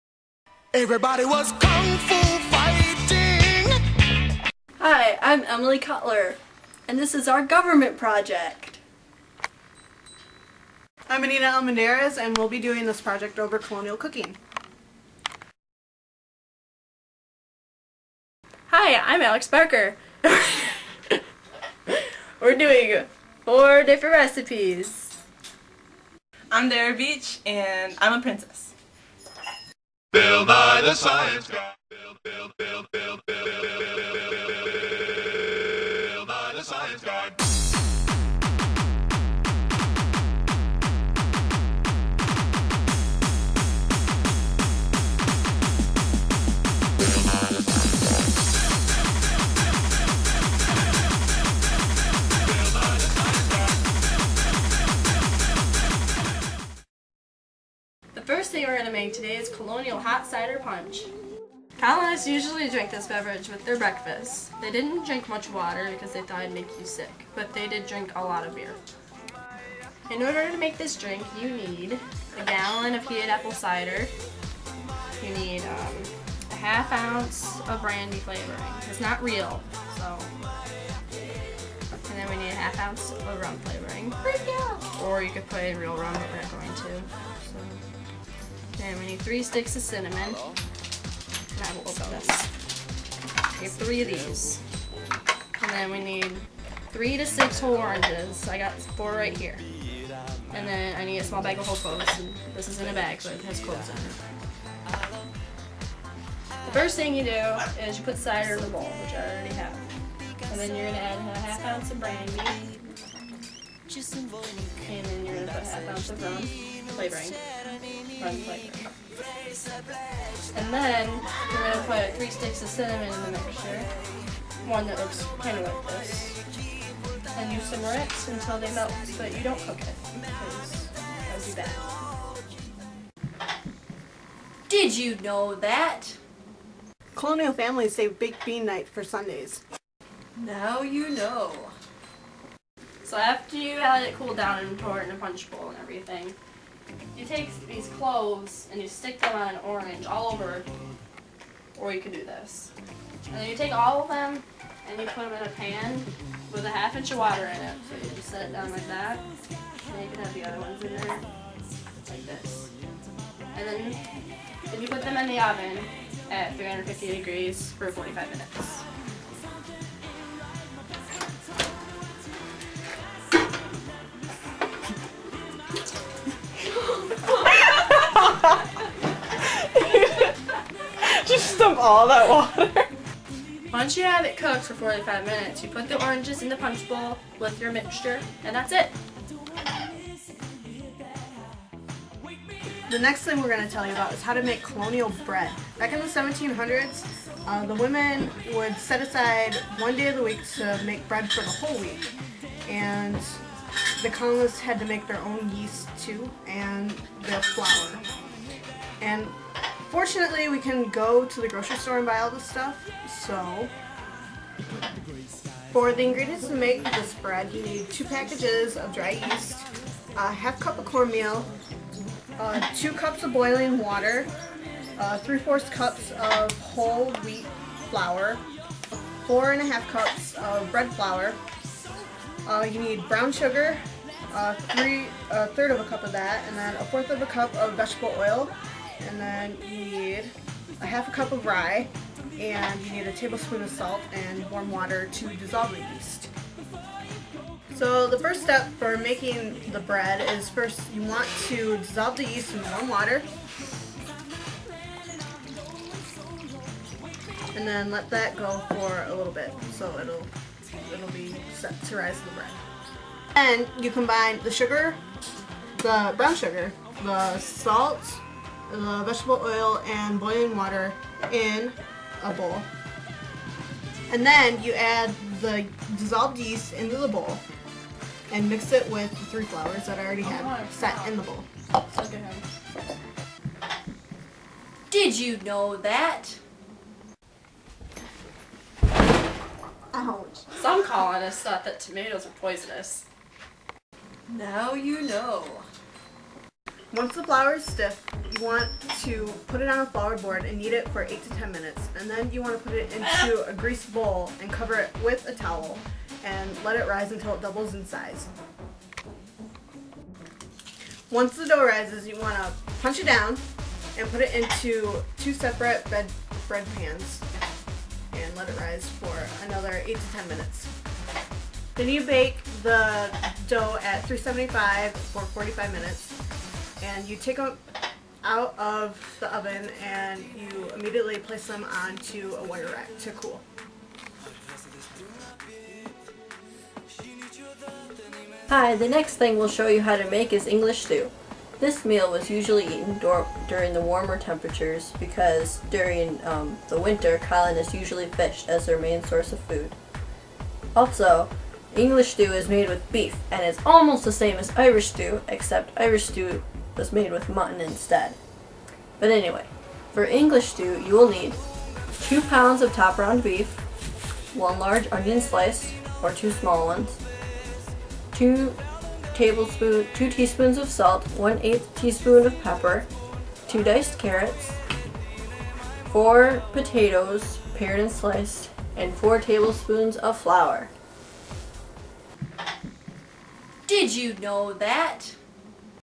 Colonial Cooking Pt1.wmv
This was our Senior Government Project
Starring Alex, Anita, Dara and Emily
Edited by Alex
It's about cooking. And colonies.
It's pretty awesome.
Yeah we got and A.